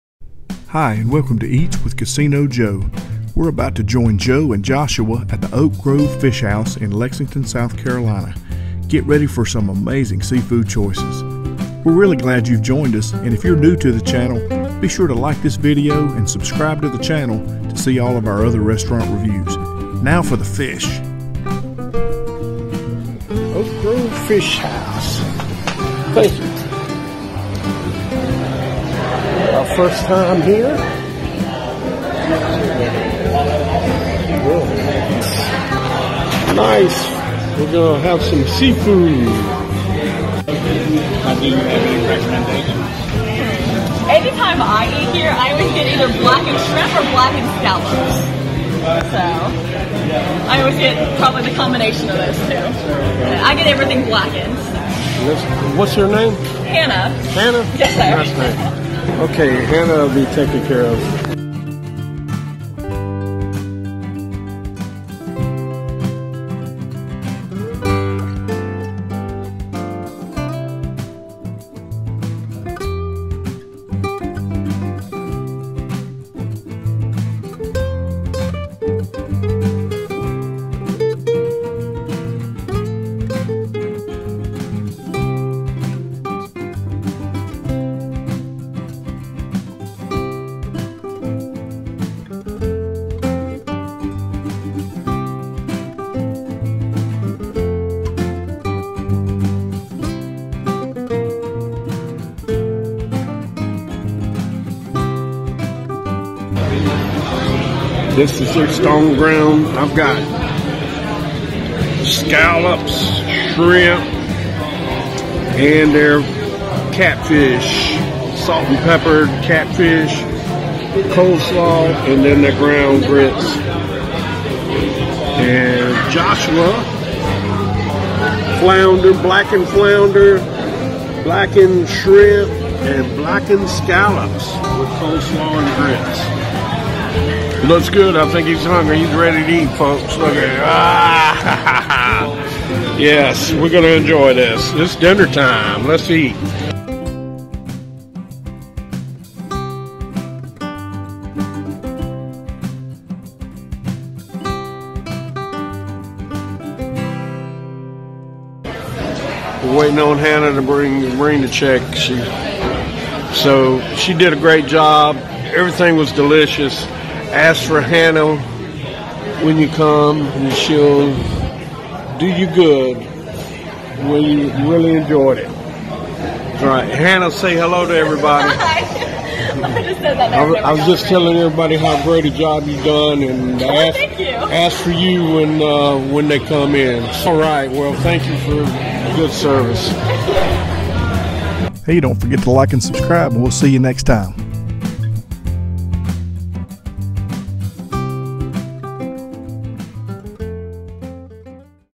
Oak Grove Fish House Lexington, SC - Restaurant Review
Casino Joe and son Joshua review the Oak Grove Fish House in Lexington, South Carolina.  Conveniently located in the Columbia metro area, it's a great seafood stop for travelers and locals alike.  They have a wide variety of seafood and other food types, along with a side of good old fashioned southern hospitality.

Let's come along as the guys chow down on some good old southern seafood classics.